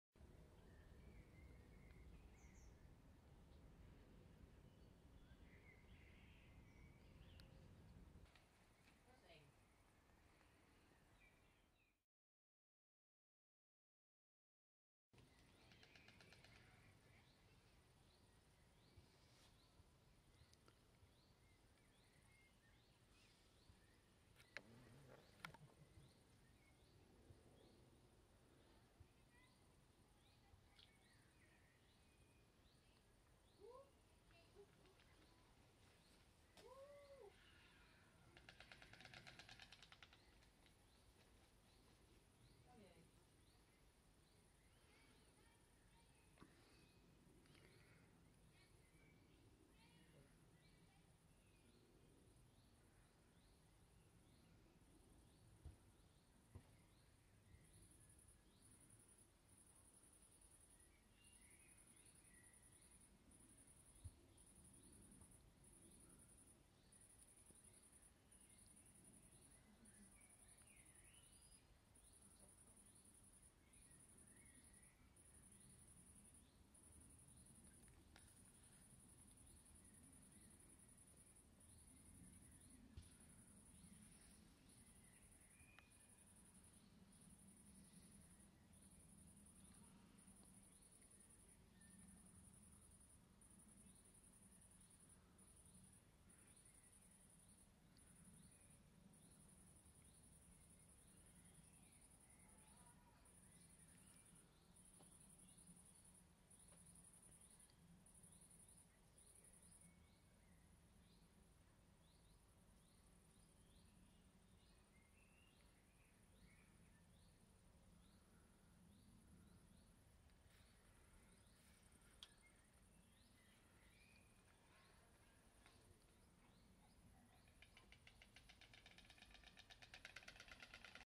Bigbroeder 18 juli 2018
Ooievaars komen terug op nest